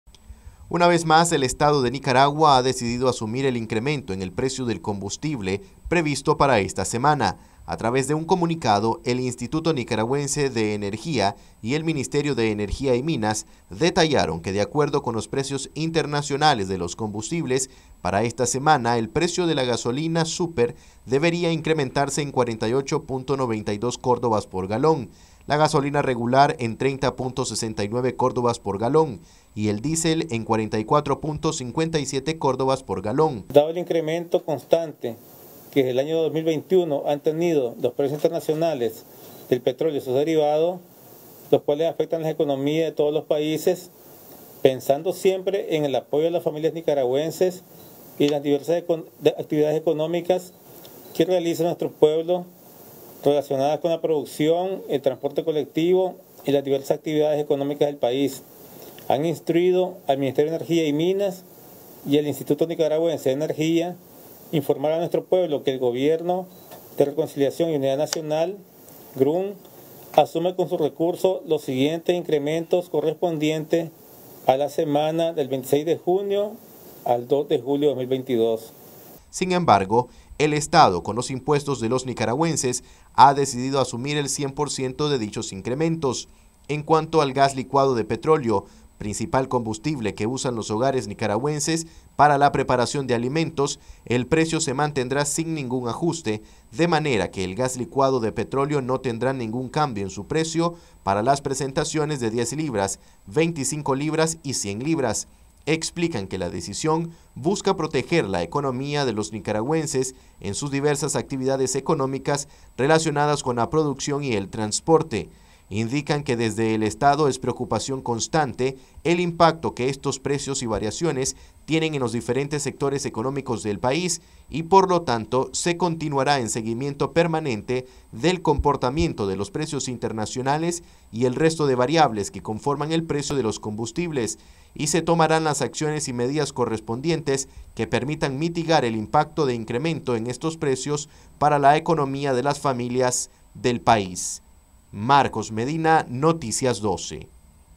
Se mantienen los precios de los combustibles en Ncaragua
Noticias12Nicaragua Se mantienen los precios de los combustibles en Ncaragua.